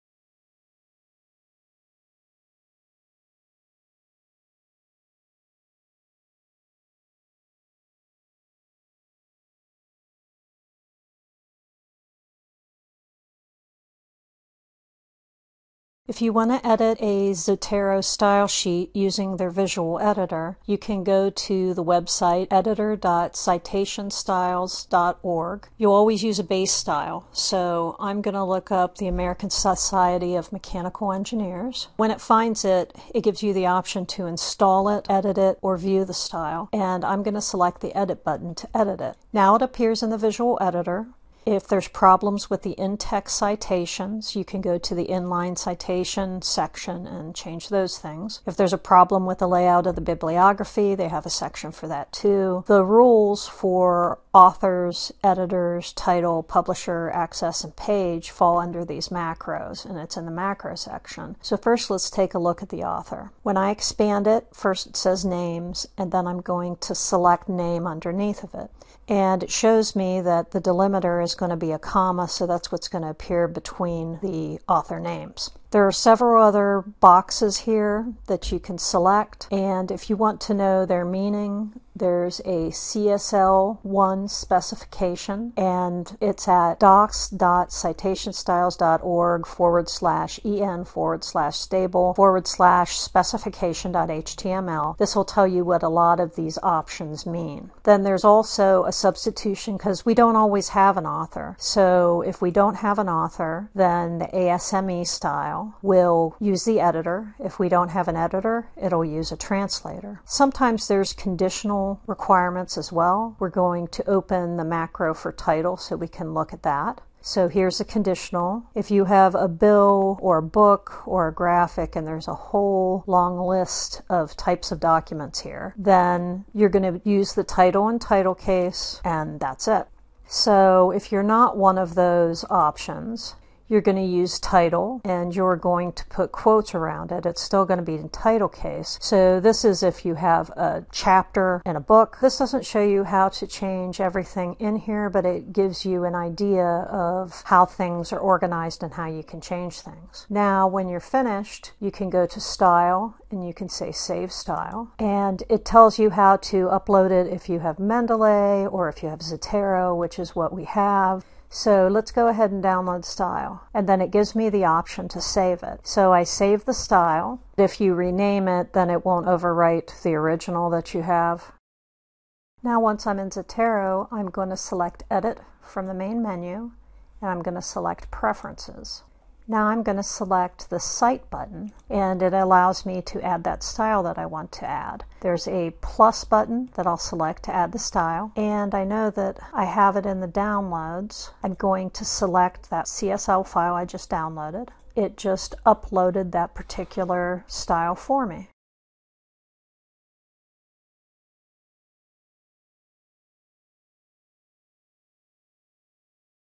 Editing a Citation Style Sheet for Zotero
Learn how to do minor editing of a citation style and load it into Zotero.